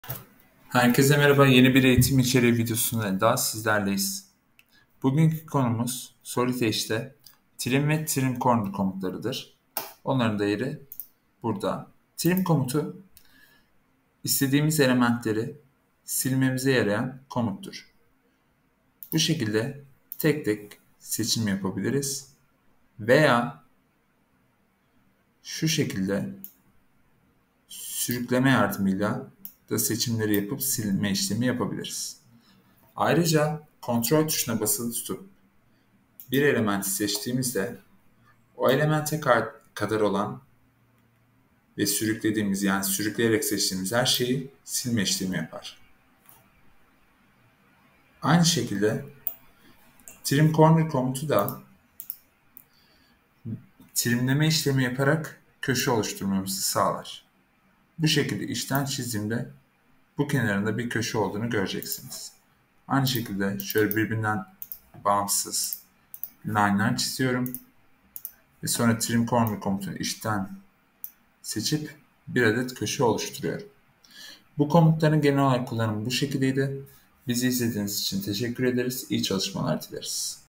Solid Edge 2023 Eğitim Videoları -Sketch Komutları-7-Trim Ve TrimCorner (Kırpma Komutları)
BU videoda Solid Edge 2023 versiyonunda kırpma komutlarını göreceksiniz.